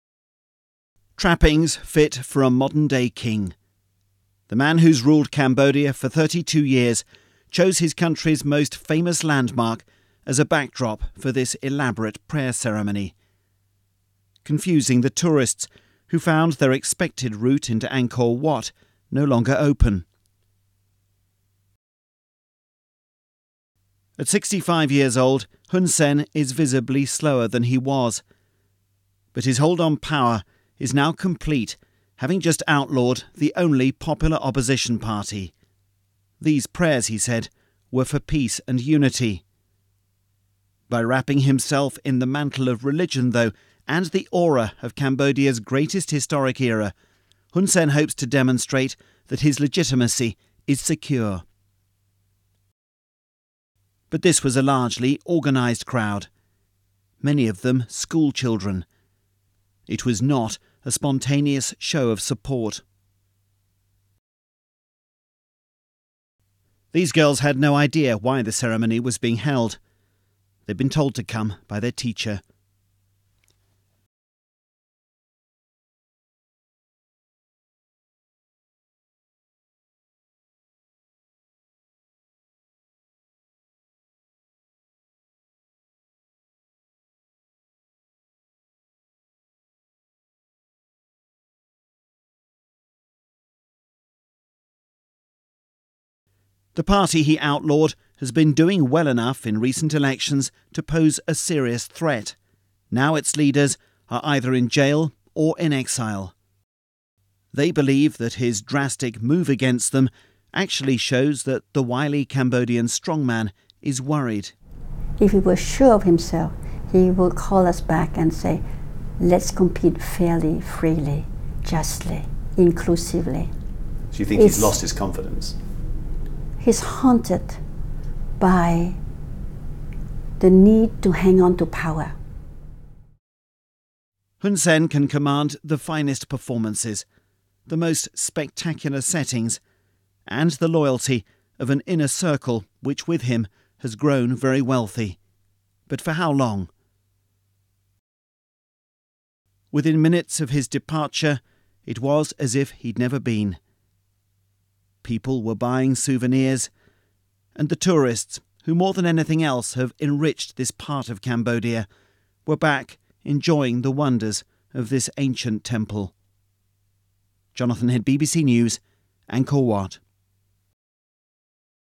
Cambodia Angkor Hun Sen Ceremony
This was fun - battling Hun Sen's aggressive bodyguards to film his extraordinary and extravagant ceremony to show national unity at Angkor Wat a year ago.